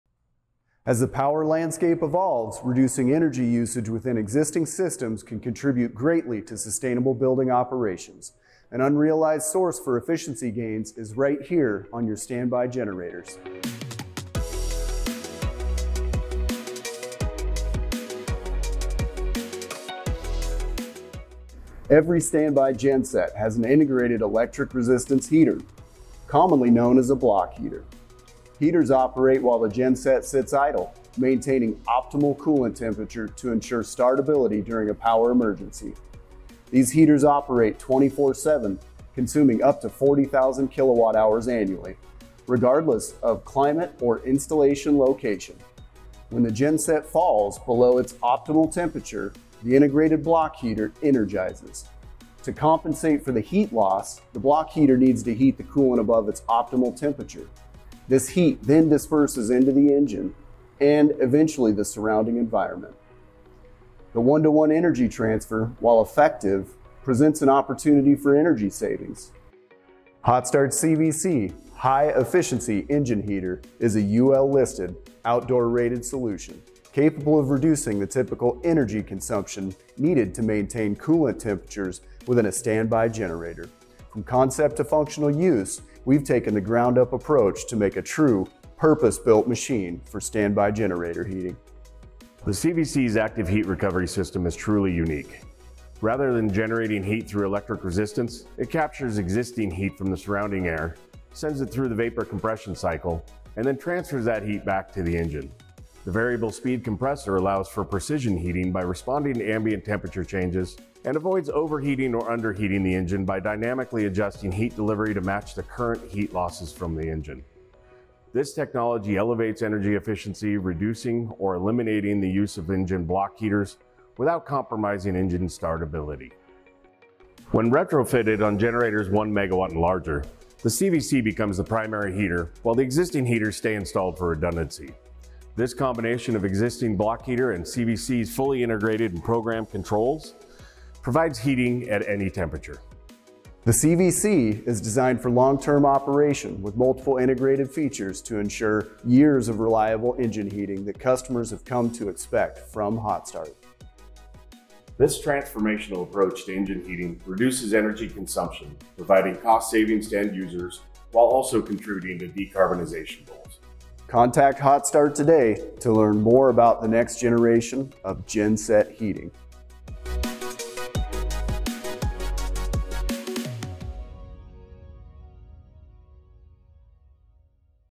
Hotstart CVC Energy Efficient Engine Heater
As the power landscape evolves, reducing energy usage within existing systems can contribute greatly to sustainable building operations. An unrealized source for efficiency gains is right here on your standby generator.

Every standby genset has an integrated electric resistance heater, commonly known as a block heater. Heaters operate while the genset sits idle, maintaining optimal coolant temperature to ensure startability during a power emergency. These heaters operate 24/7, consuming up to 40,000 kilowatt hours annually regardless of climate or installation location.

When the genset falls below its optimal temperature, the integrated block heater energizes. To compensate for the heat loss, the block heater needs to heat the coolant above its optimal temperature. This heat then disperses into the engine block and eventually the surrounding environment. The one-to-one energy transfer, while effective, presents an opportunity energy savings.

Hotstart’s CVC high efficiency engine heater is a UL listed; outdoor rated solution capable of reducing the typical energy consumption needed to maintain coolant temperatures within a standby generator. From concept to functional use, we’ve taken the ground up approach to make a true purpose-built machine for standby generator heating.

The CVC’s active heat recovery system is truly unique; rather than generating heat through electric resistance, it captures the existing heat from the surrounding air, sends it through the vapor compression cycle, and then transfers that heat back into the engine. The variable speed compressor allows for precision heating by responding to ambient temperature changes and avoids overheating or underheating the engine by dynamically adjusting heat delivery to match the current heat losses from the engine. This technology elevates energy efficiency, reducing or eliminating the use of the existing block heaters without compromising engine startability.

When retrofitted on generators 1 megawatt and larger, the CVC becomes the primary heater while the existing engine heaters stay installed for redundancy. This combination of the existing block heater and the CVC’s fully integrated and programmed controls provides heating at any temperature.

The CVC is designed for long-term operation with multiple integrated features to ensure years of reliable engine heating that customers have come to expect from Hotstart.

This transformational approach to engine heating reduces energy consumption, providing energy and cost savings to end-users while also contributing to decarbonization goals.

Contact Hotstart today to learn more about the next generation of genset heating.